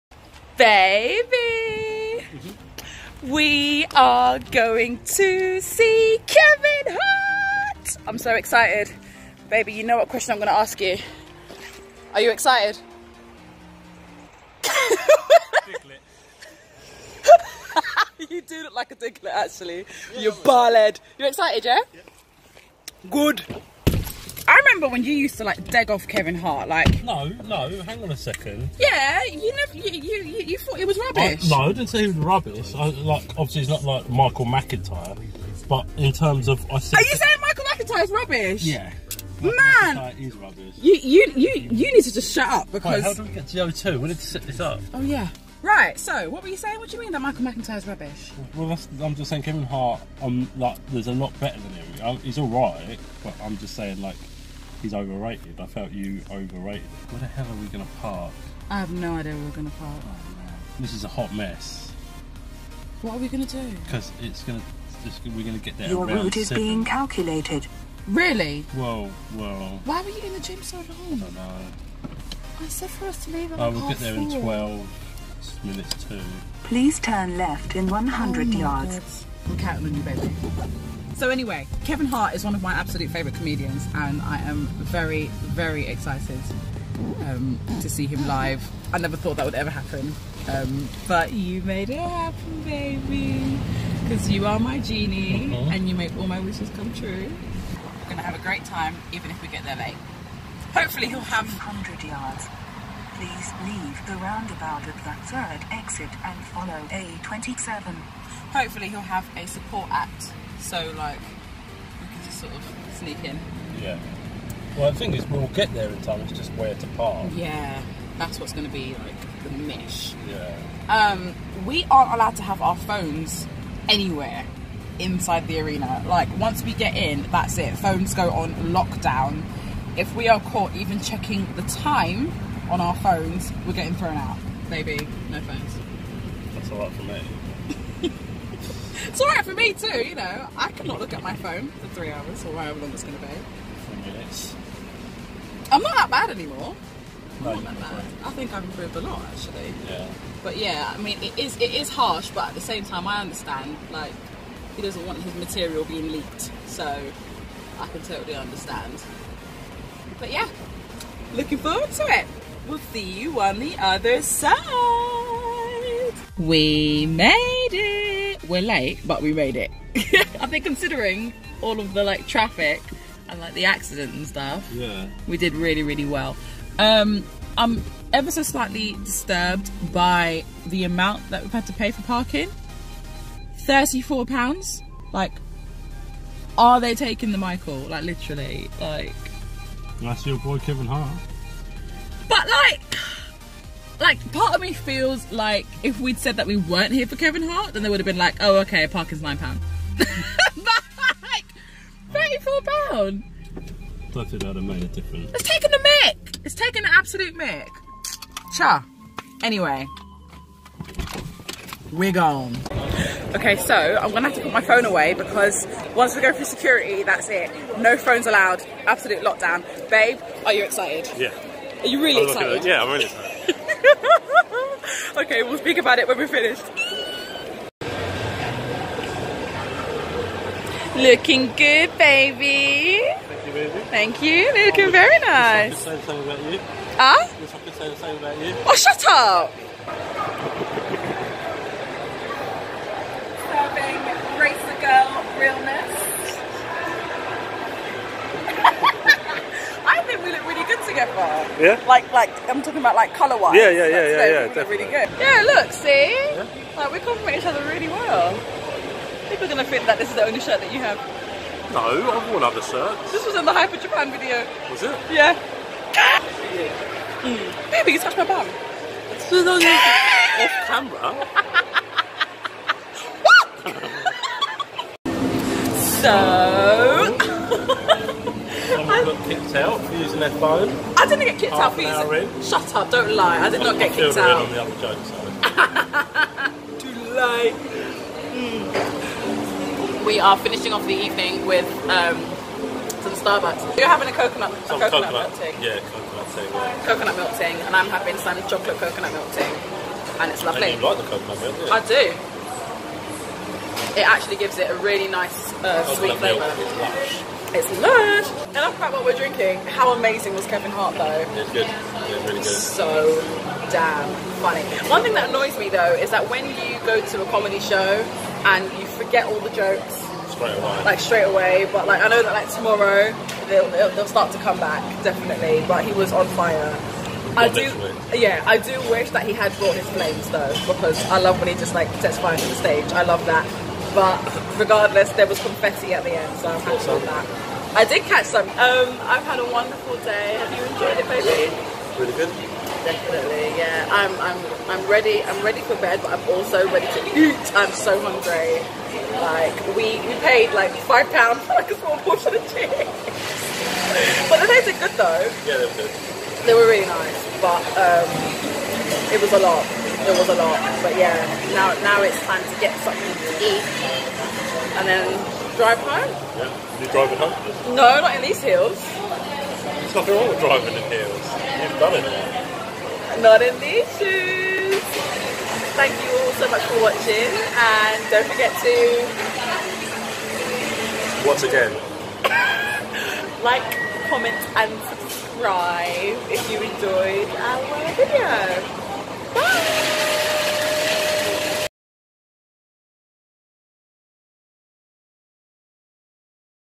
Ayesha And Mike - Kevin Hart Irresponsible Tour Vlog
Eeeeee! One of my favourite comedians ever! I was so excited to see him live! We really enjoyed ourselves!